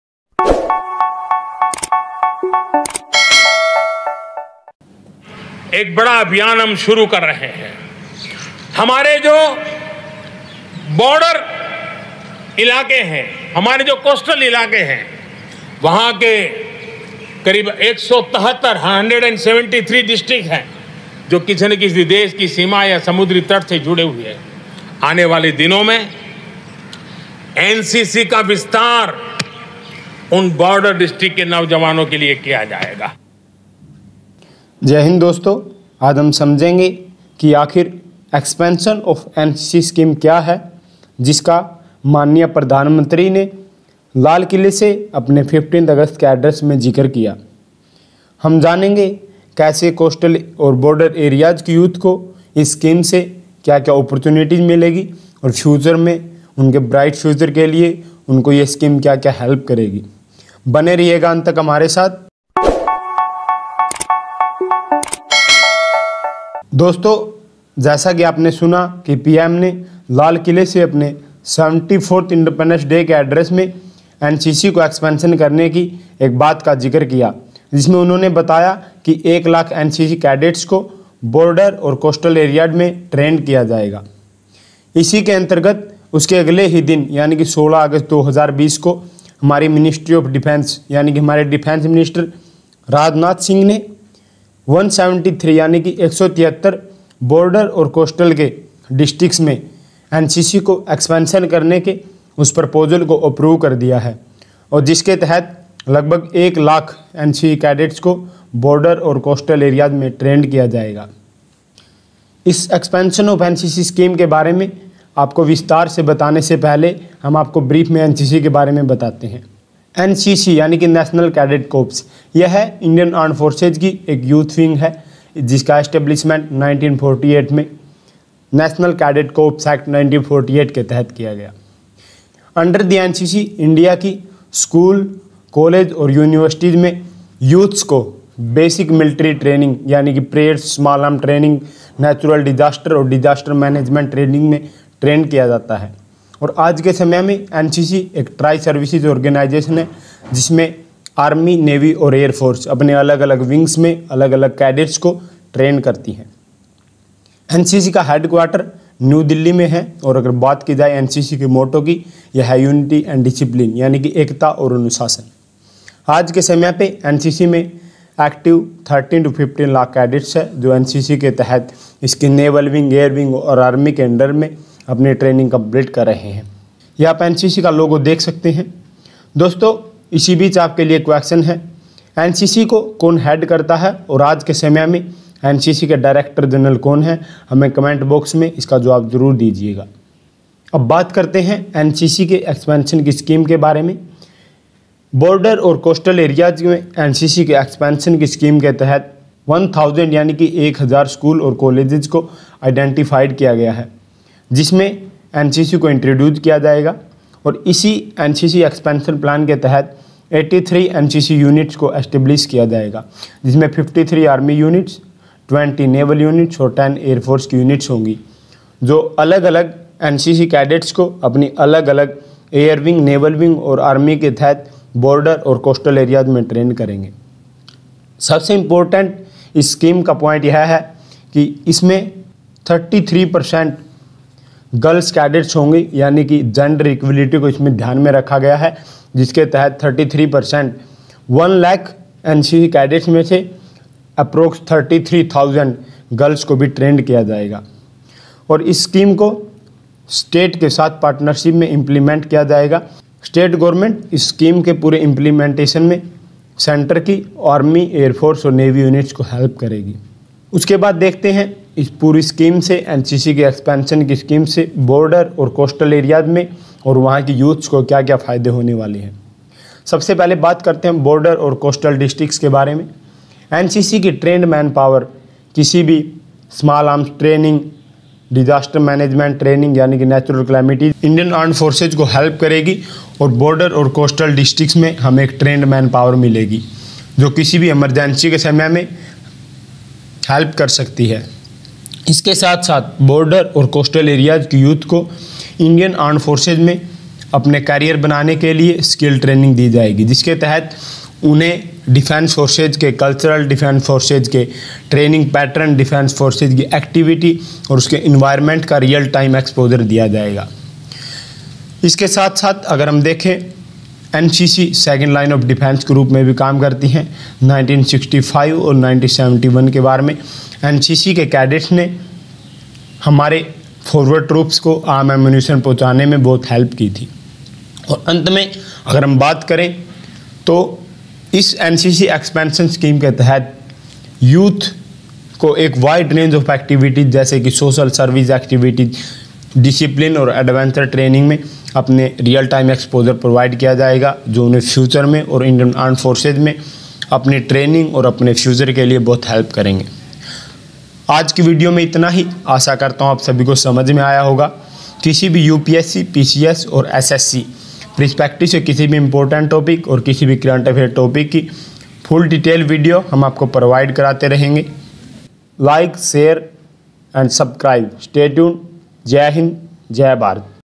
National Cadet Corps (NCC) Expansion , One (01) Lakh Cadet To Be Trained | UPSC | PCS | SSB |DEFENSE
FULL ANALYSIS OF NCC EXPANSION IN 173 COASTAL AND BORDER AREAS BY GOVT OF INDIA TO TRAINED 01 LAKH CADETS .